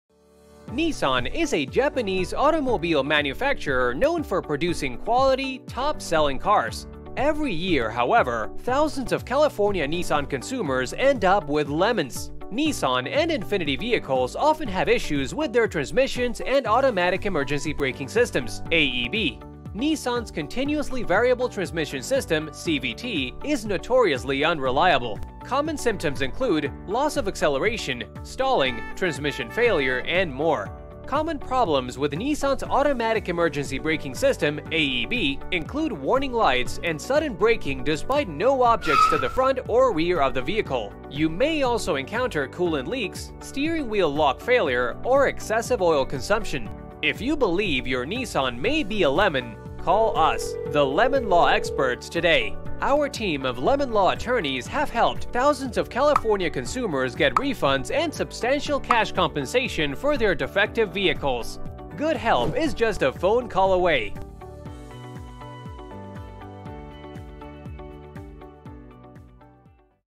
Nissan CVT Transmission Problems, Collision Warning and Other Defects
Nissan and Infiniti are notorious for their defective CVT Transmission and forward emergency braking collision warning system. Thousands of consumers in California have ended up with lemon vehicles plagued by these defects along with various others.

If you have problems with your Nissan, give us a call today, or visit us